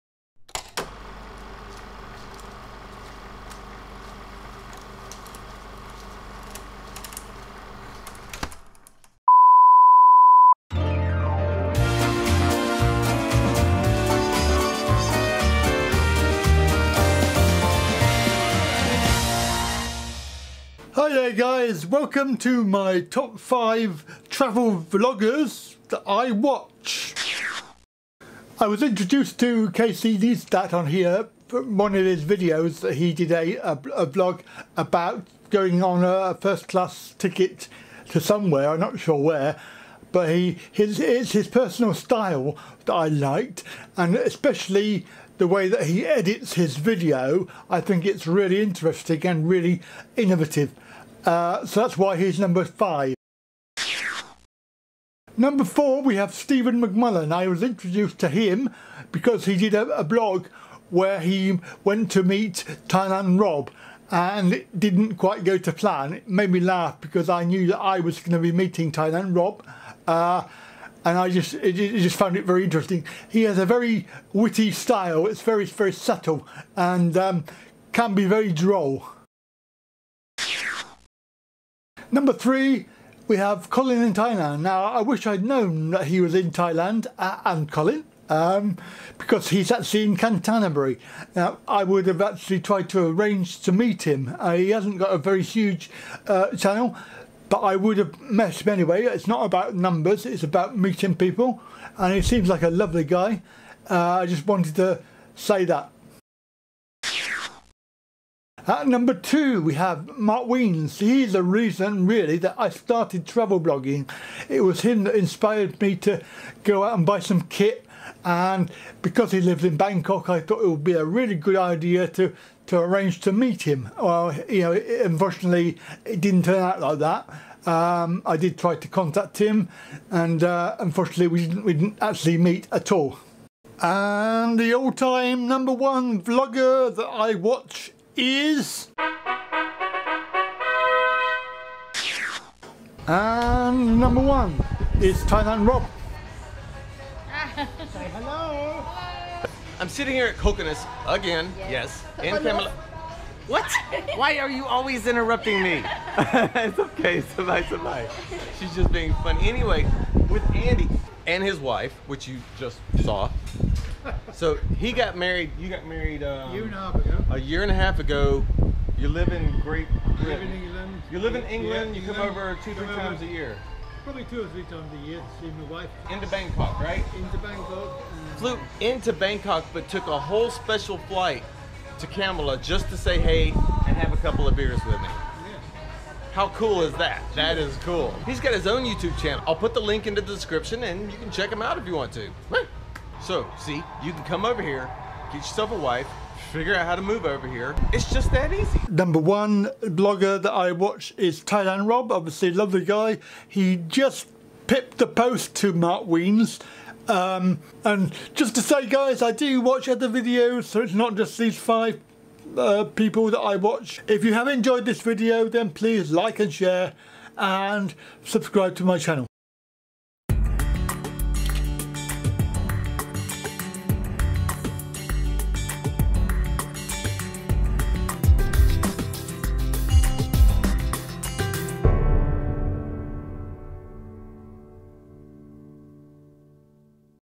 My Top Five - The Top 5 Travel Vloggers I Watch on Youtube
Here's my personal top five vloggers I enjoy watching when I am not making my own videos.   There are many more I watch, but these are the main ones for now, and 4 out of 5 are in or about Thailand!

CREDIT ROLL
or using Creative Commons Licences.  Sound effects via various
and by snail mail to the address on my website contact page.